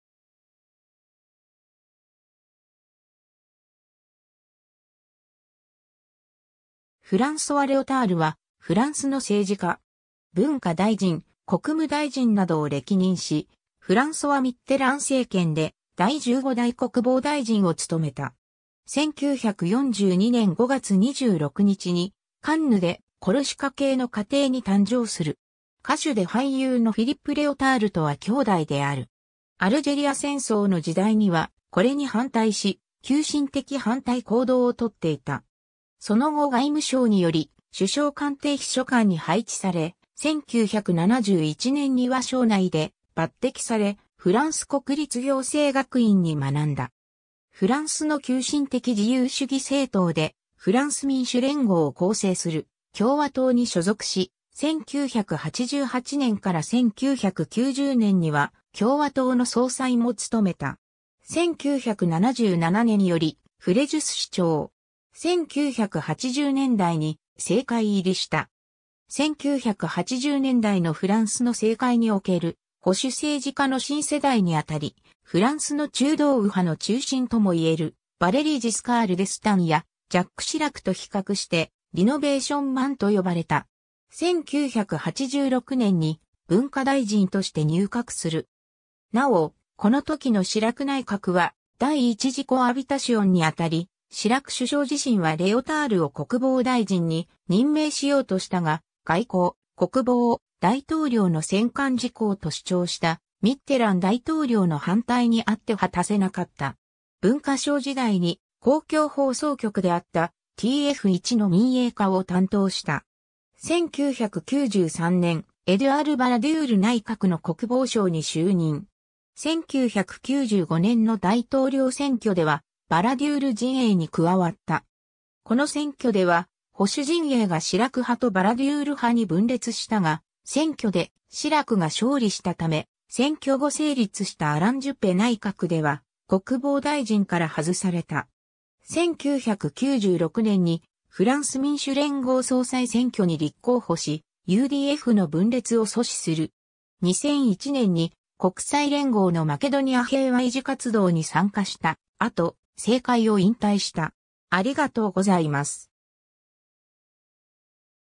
フランソワ・レオタール
フランソワ・レオタール（フランス語: François Léotard、1942年5月26日 - ）は、フランスの政治家。
文化大臣、国務大臣などを歴任し、フランソワ・ミッテラン政権で第15代国防大臣を務めた。
 1942年5月26日にカンヌでコルシカ系の家庭に誕生する。
歌手で俳優のフィリップ・レオタールとは兄弟である。
アルジェリア戦争の時代には、これに反対し急進的反対行動をとっていた。
その後外務省により、首相官邸秘書官に配置され、1971年には省内で抜擢されフランス国立行政学院（ENA）に学んだ。
 フランスの急進的自由主義政党で、フランス民主連合（UDF）を構成する共和党に所属し、1988年から1990年には共和党の総裁も務めた。
1977年よりフレジュス市長（1997年に辞職）。
1980年代に政界入りした。
1980年代のフランスの政界における保守政治家の新世代に当たり、フランスの中道右派の中心ともいえる、ヴァレリー・ジスカール・デスタンやジャック・シラクと比較して、リノヴェーションマン（刷新者）と呼ばれた。
 1986年に文化大臣として入閣する。
なお、この時のシラク内閣は第1次コアビタシオンに当たり、シラク首相自身はレオタールを国防大臣に任命しようとしたが、外交・国防を大統領の専管事項と主張したミッテラン大統領の反対にあって果たせなかった。
文化相時代に公共放送局であったTF1の民営化を担当した。
1993年エドゥアール・バラデュール内閣の国防相に就任。
1995年の大統領選挙ではバラデュール陣営に加わった。
この選挙では保守陣営がシラク派とバラデュール派に分裂したが、選挙でシラクが勝利したため、選挙後成立したアラン・ジュペ内閣では国防大臣から外された。
1996年にフランス民主連合総裁選挙に立候補し、UDFの分裂を阻止する。
2001年に国際連合のマケドニア平和維持活動に参加した後、政界を引退した。